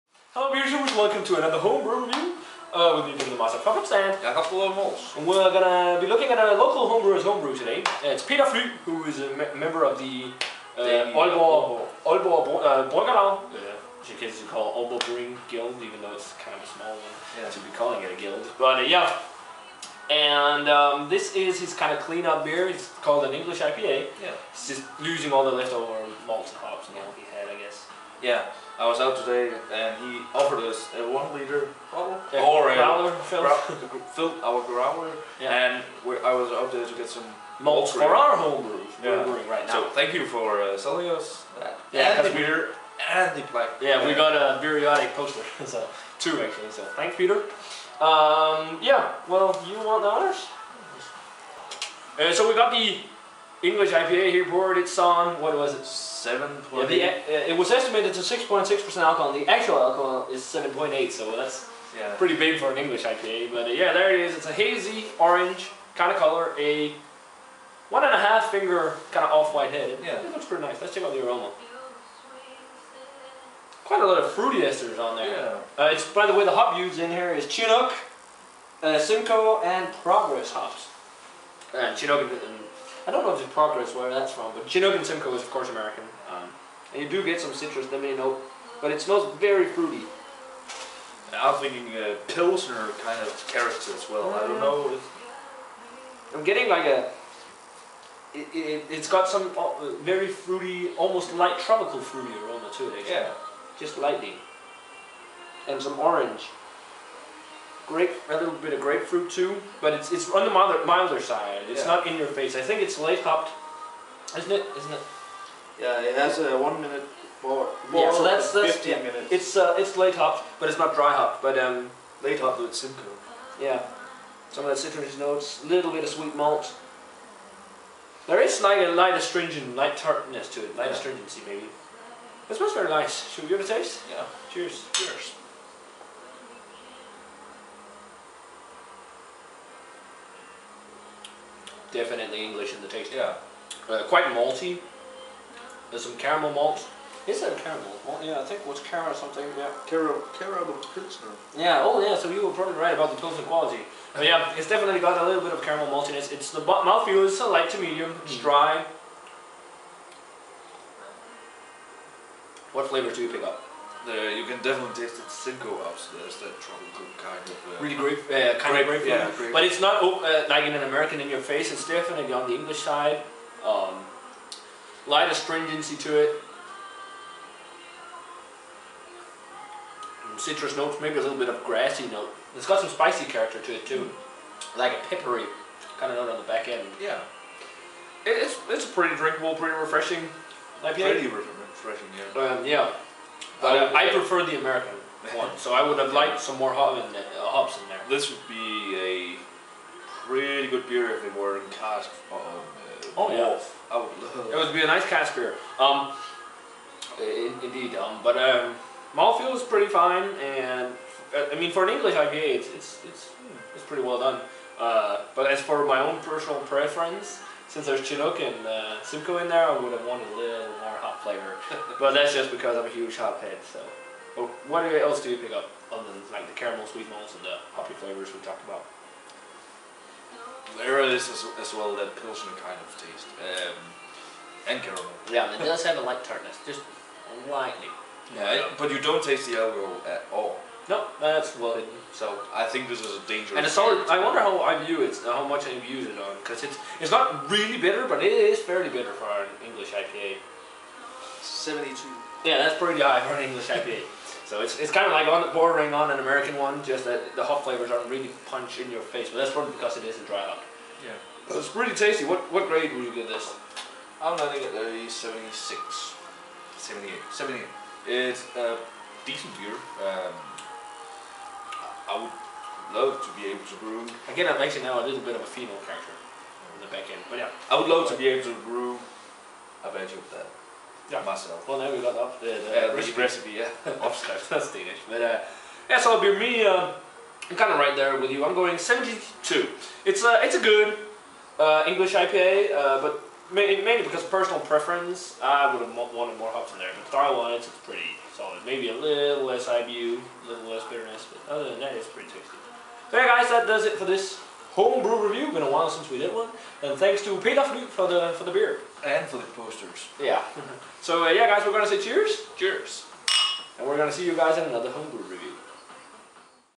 TMOH - Homebrew Review 5#: Peter Fly's Engelsk IPA
This homebrew is from a local homebrewer called Peter Fly, who is a part of the local homebrewers guild. Its an English style IPA on 7,8%.

Check out the review to see what me and Jakob thought.